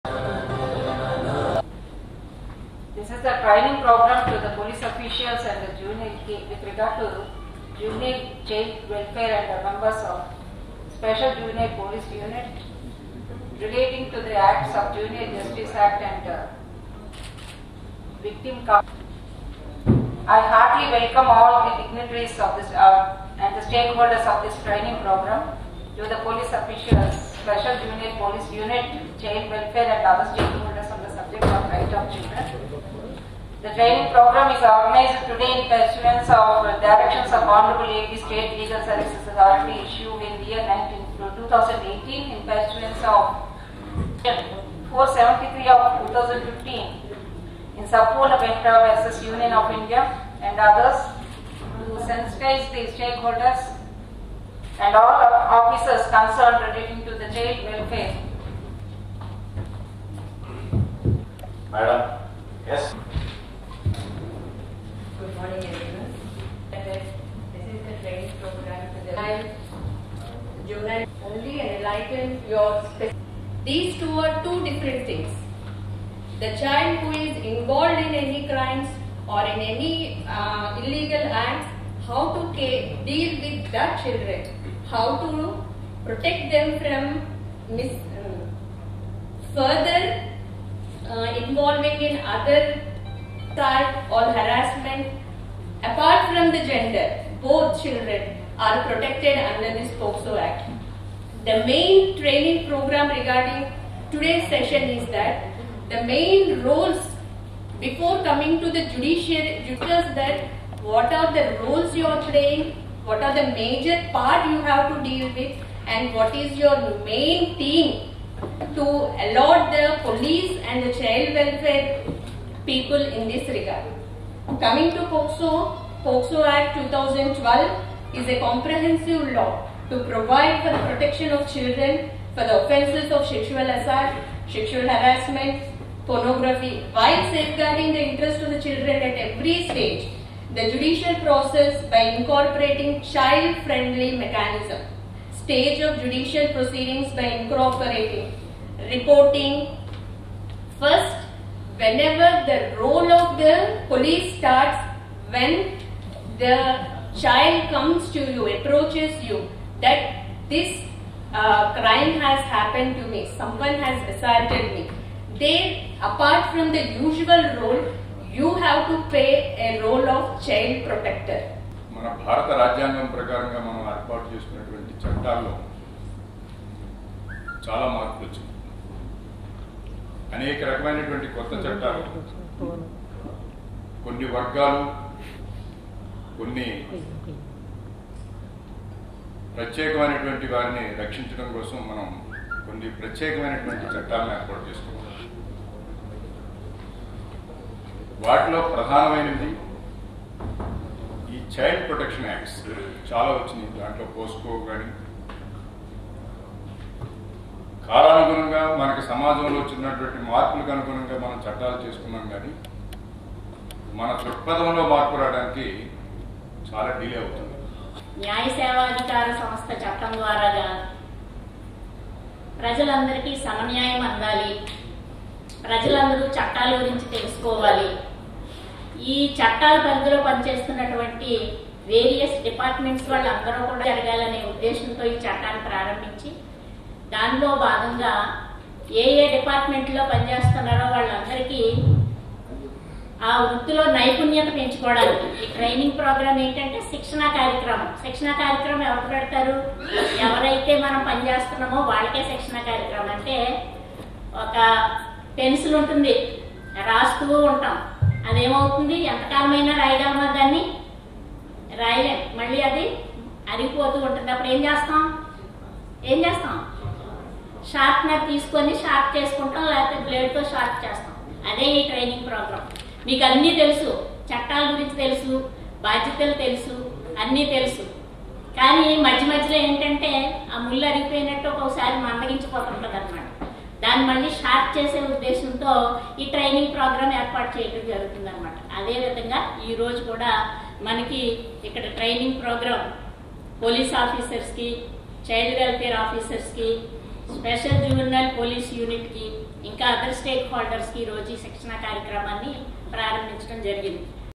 4.3.23_ELR_Training Programme To The Police Officers,Juvenile officer_smt P.Manga kumari
4.3.2023_ELR_Training Programme To The Police Officers,Juvenile child welfare officers_smt P Manga kumari.I Addl.District Judge,W.G,ELR
Training Programme
smt P Manga kumari.I Addl.District Judge,W.G,ELR